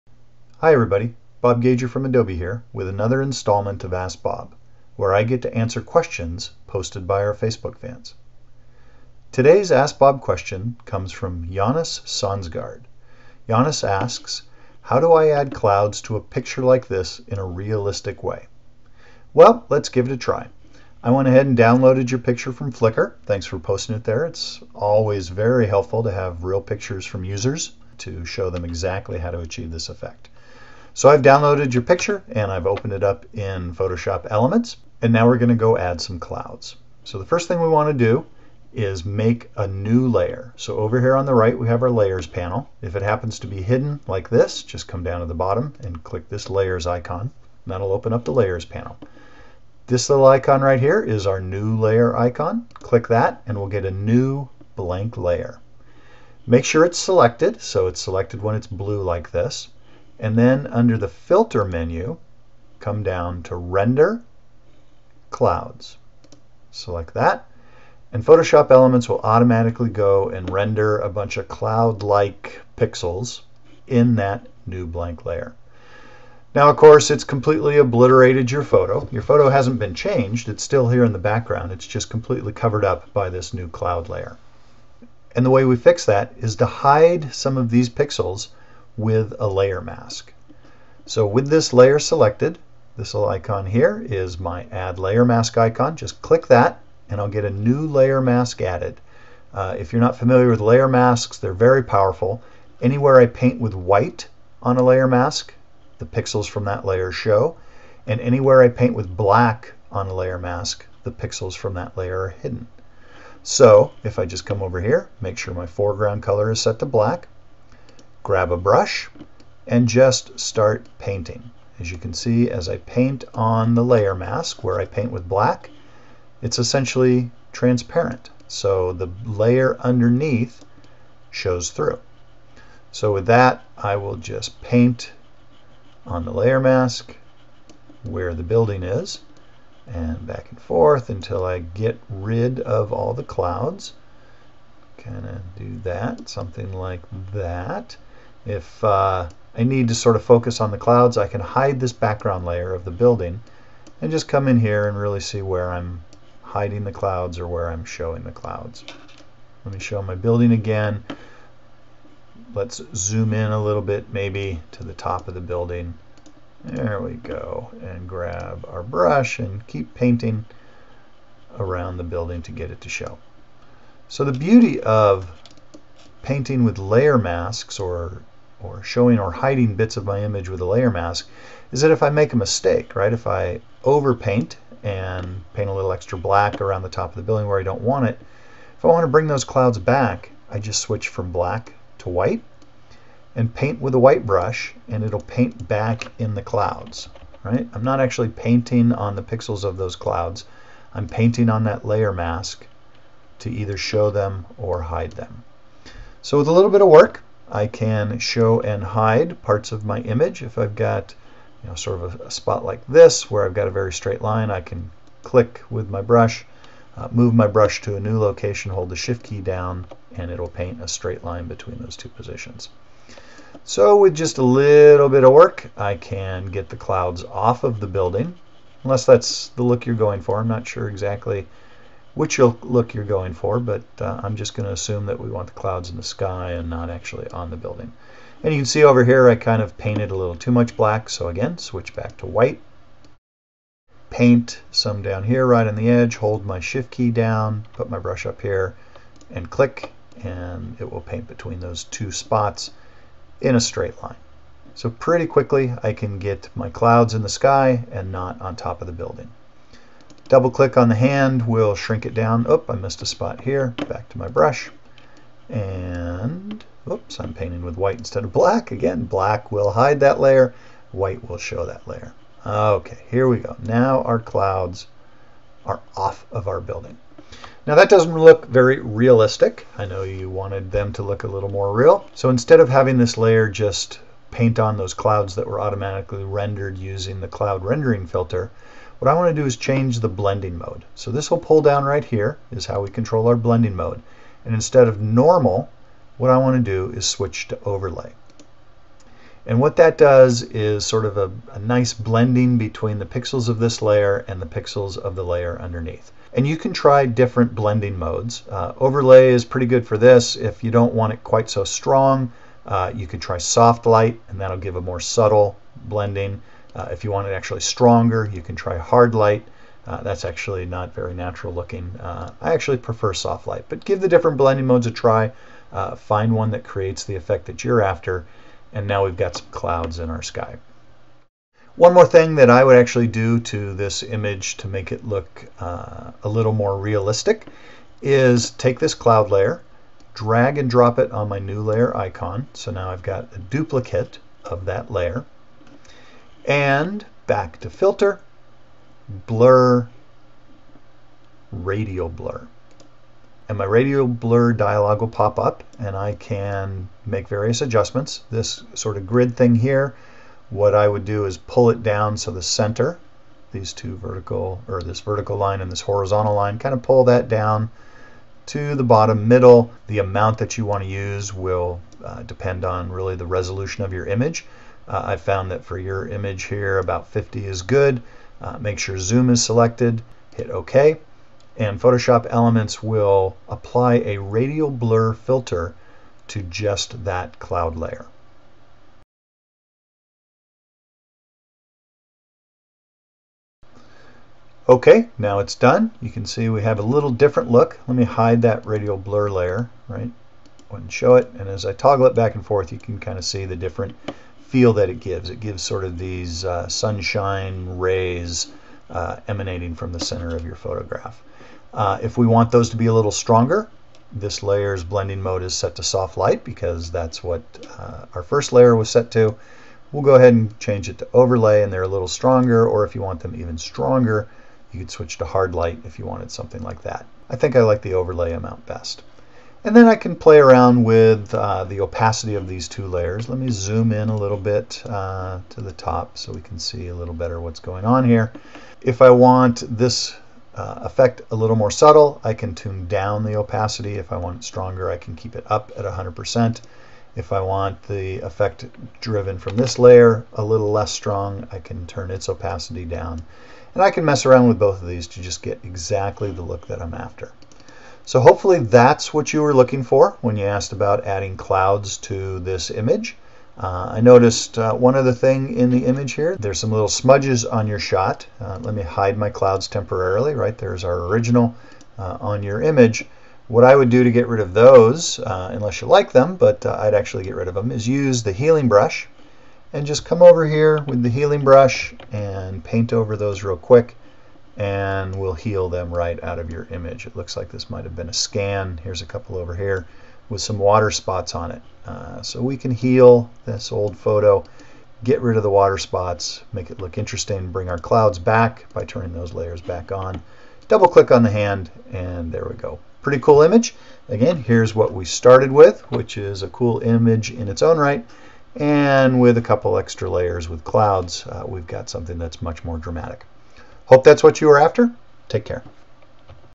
Adding Clouds in Photoshop Elements 11
Bob Gager, Group Product Manager for Photoshop Elements, responds to a fan question explaining how to add clouds to photos in Photoshop Elements 11.